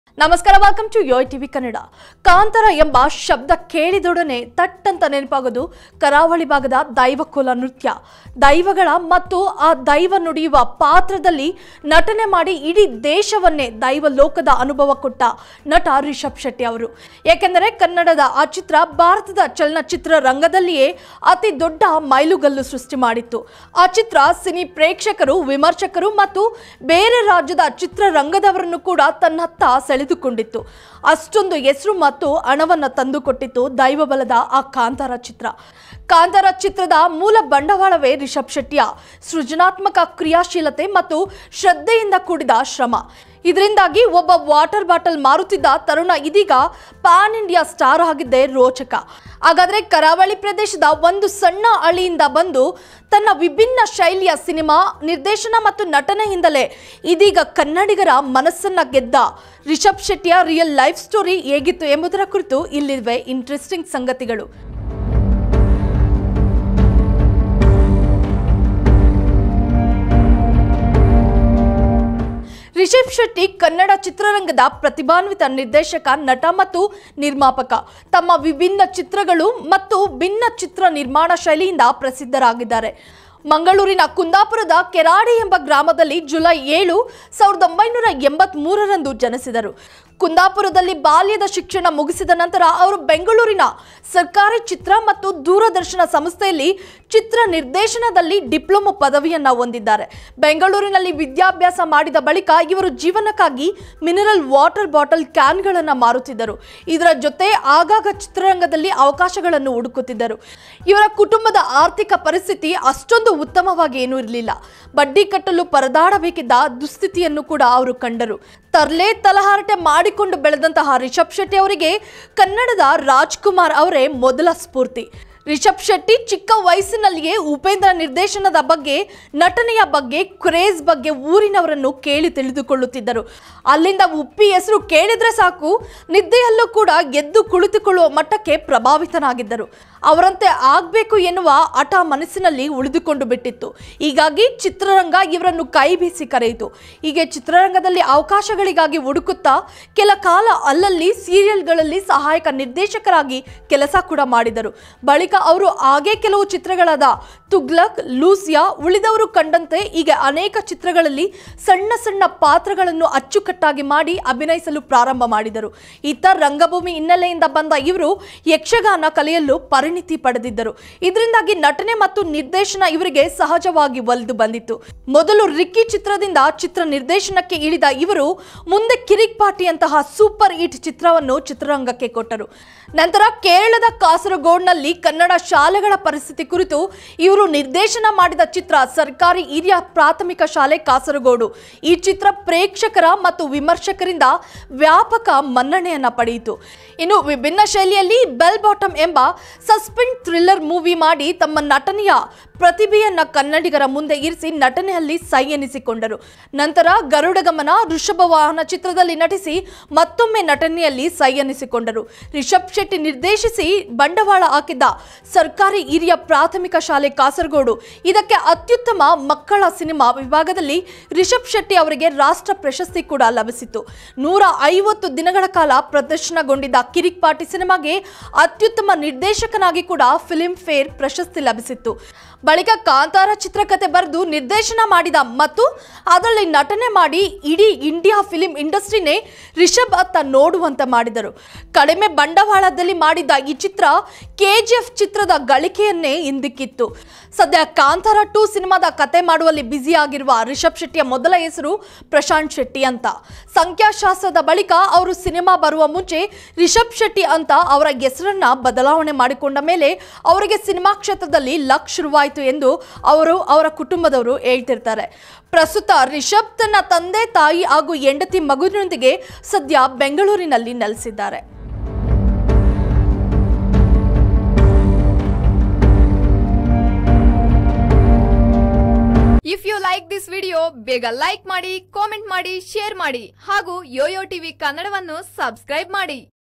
Actor Rishab Shetty Life Story ( Biography ) | Family | Education | Cine Journey |YOYO TV Kannada Bi
ಕಾಂತಾರ ಎಂಬ ಶಬ್ದ ಕೇಳಿದೊಡನೆ ಥಟ್ಟ ಅಂತಾ ನೆನಪಾಗೋದು ಕರಾವಳಿ ಭಾಗದ ದೈವ ಕೋಲ ನೃತ್ಯ, ದೈವಗಳು ಮತ್ತು ಆ ದೈವ ನುಡಿಯುವ ಪಾತ್ರದಲ್ಲಿ ನಟನೆ ಮಾಡಿ ಇಡೀ ದೇಶವನ್ನೇ ದೈವಲೋಕದ‌ ಅನುಭವ ಕೊಟ್ಟ ನಟ ರಿಷಬ್ ಶೆಟ್ಟಿ.. ಏಕೆಂದರೆ ಕನ್ನಡದ ಆ ಚಿತ್ರ  ಭಾರತದ ಚಲಚಿತ್ರದ ಇತಿಹಾಸದಲ್ಲಿಯೇ ಅತಿ ದೊಡ್ಡ ಮೈಲುಗಲ್ಲು ಸೃಷ್ಟಿ ಮಾಡಿದ್ದು. ಆ ಚಿತ್ರದ ಸಿನಿ ಪ್ರೇಕ್ಷಕರು, ವಿಮರ್ಶಕರು, ಮತ್ತು ಬೇರೆ ರಾಜ್ಯದ ಚಿತ್ರರಂಗದವರನ್ನು ಕೂಡಾ ತನ್ನ ಅತ್ತ ಸೆಳೆದಿತ್ತು. ಅಷ್ಟೊಂದು ಹೆಸರು ಮತ್ತು ಹಣವನ್ನು ತಂದು ಕೊಟ್ಟಿತ್ತು ದೈವಬಲದ ಆ ಕಾಂತರ.

ಆ ಕಾಂತರ ಚಿತ್ರದ ಮೂಲ ಬಂಡವಾಳವೇ ರಿಷಬ್ ಶೆಟ್ಟಿ ಯ ಸೃಜನಾತ್ಮಕ ಕ್ರಿಯಾಶೀಲತೆ ಮತ್ತು ಶ್ರದ್ಧೆಯಿಂದ ಕೂಡಿದ ಶ್ರಮ. ಇದರಿಂದಾಗಿ ಒಬ್ಬ ವಾಟರ್ ಬಾಟಲ್ ಮಾರುತ್ತಿದ್ದ ತರುಣ ಇದೀಗ ಪ್ಯಾನ್ ಇಂಡಿಯಾ ಸ್ಟಾರ್ ಆಗಿದ್ದೇ ರೋಚಕ.‌ ಹಾಗದ್ರೆ ಕರಾವಳಿ ಪ್ರದೇಶದ ಒಂದು ಸಣ್ಣ ಹಳ್ಳಿಯಿಂದ ಬಂದು ತನ್ನ ವಿಭಿನ್ನ ಶೈಲಿಯ ಸಿನಿಮಾ ನಿರ್ದೇಶನ ಮತ್ತು ನಟನೆಯಿಂದಲೇ ಇದೀಗ ಕನ್ನಡಿಗರ ಮನೆ ಮನೆ ಮಗನಾದ ರಿಷಬ್ ಶೆಟ್ಟೆಯ ರಿಯಲ್ ಲೈಪ್ ಸ್ಟೋರಿ ಹೇಗಿತ್ತು ಎಂಬುದರ ಕುರಿತು ಇಲ್ಲಿವೆ ನೋಡಿ ಇಂಟ್ರಸ್ಟಿಂಗ್ ಸಂಗತಿಗಳು.

ರಿ‍ಷಭ್ ಶೆಟ್ಟಿ ಕನ್ನಡ ಚಿತ್ರರಂಗದ ಪ್ರತಿಭಾನ್ವಿತ ನಿರ್ದೇಶಕ, ನಟ ಮತ್ತು ನಿರ್ಮಾಪಕ. ತಮ್ಮ ವಿಭಿನ್ನ ಚಿತ್ರಗಳು ಮತ್ತು ಭಿನ್ನ ಚಿತ್ರ ನಿರ್ಮಾಣ ಶೈಲಿಯಿಂದ ಪ್ರಸಿದ್ಧರಾಗಿದ್ದಾರೆ.